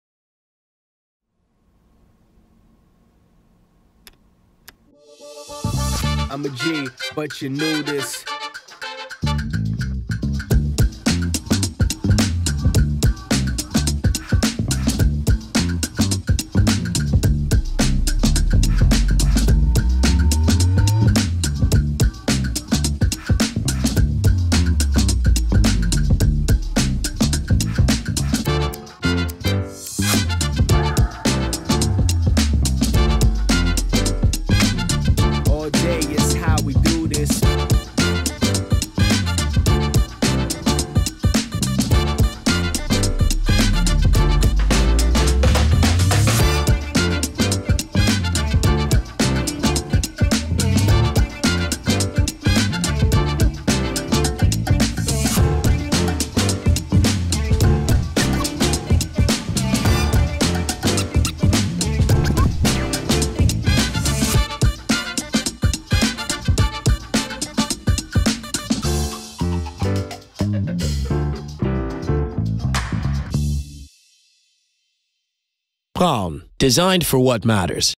ALL NEW Braun Series 7 with 360 Flexhead
Electric shaver with 360 ° adjustment for a close shave – even in hard-to-reach areas
AutoSense technology detects your beard growth and adapts the motor output to the beard density. Shaves even thick beards
Including: 4-in-1 cleaning station, EasyClick precision trimmer attachment
Lithium-ion battery for up to 3 weeks of shaving (50 minutes). A quick charge of 5 minutes is enough for a complete shave
100% waterproof electric razor for wet and dry shaving